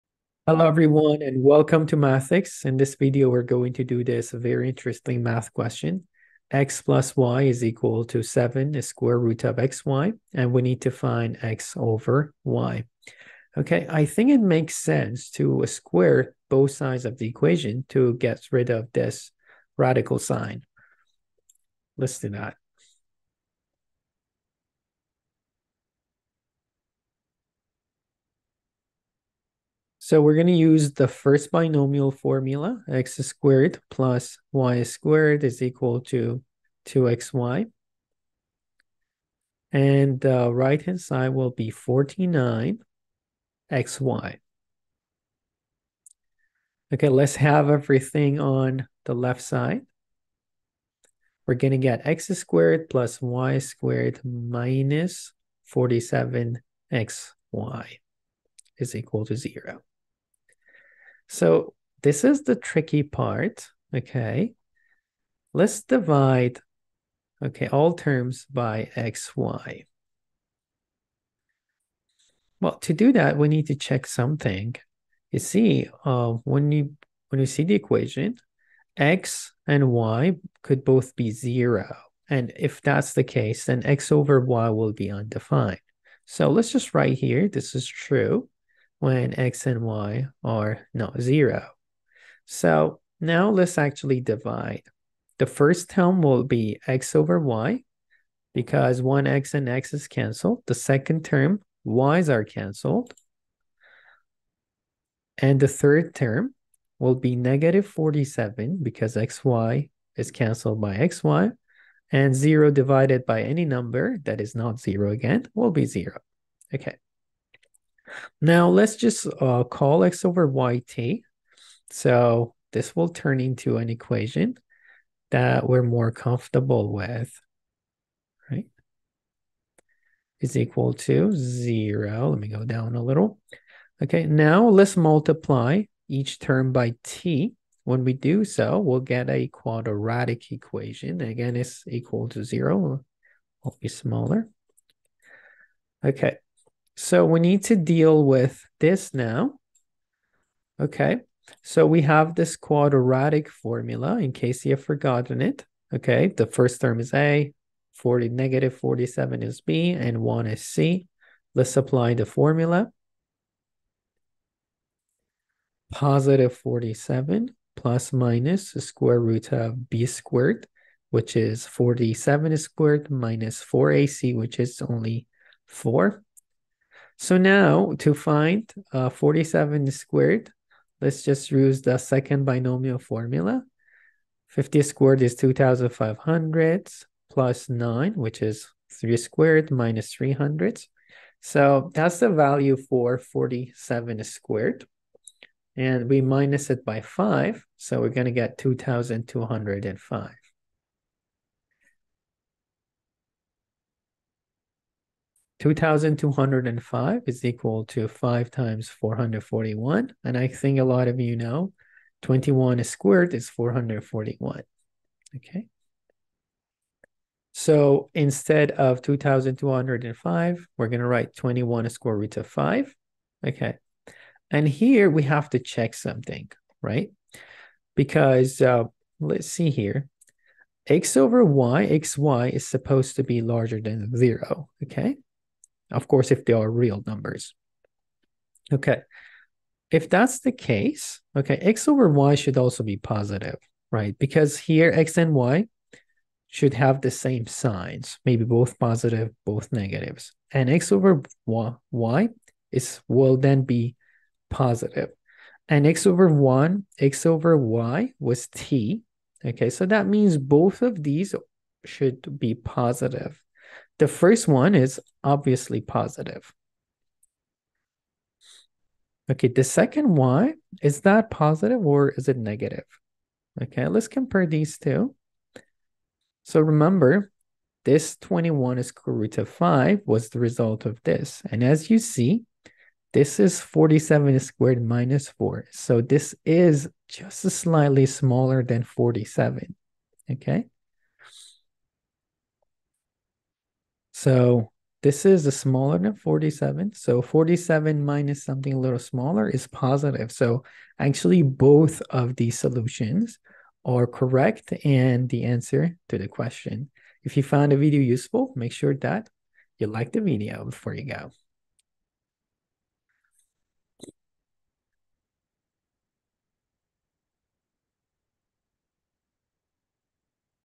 Solve this Fun Equation | 97.3% Fail to Solve this Math Problem
The questions looks deceptively easy but it's very tricky.

X + Y = 7 √XY
What is X/Y ?